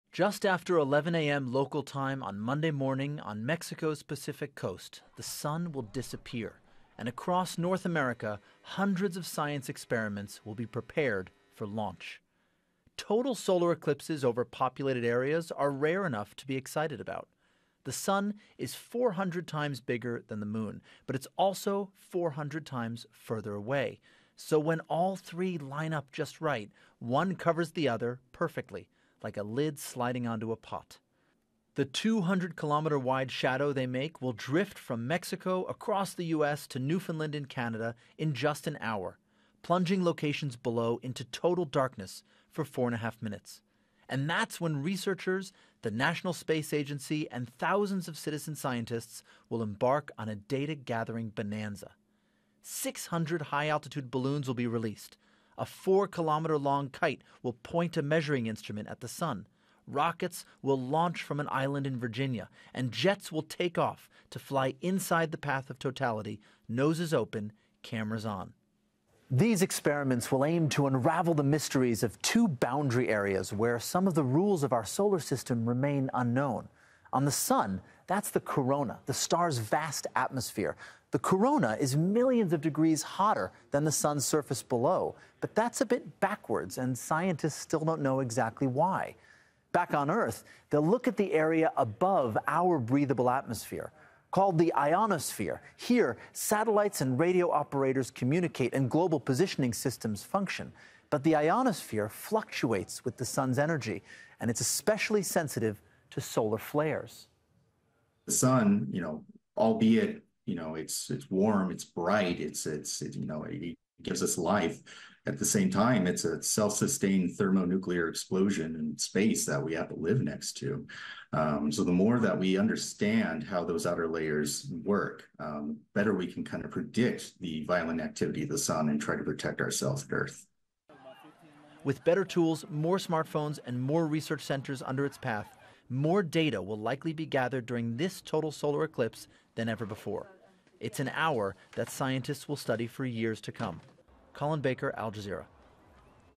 Total solar eclipse 2024: Where, when, and how to watch
Millions of people are arriving in the so-called Path of Totality across the United States, where a full solar eclipse will soon block the sun.

Al Jazeera’s Colin Baker explains how scientists are preparing for the moment.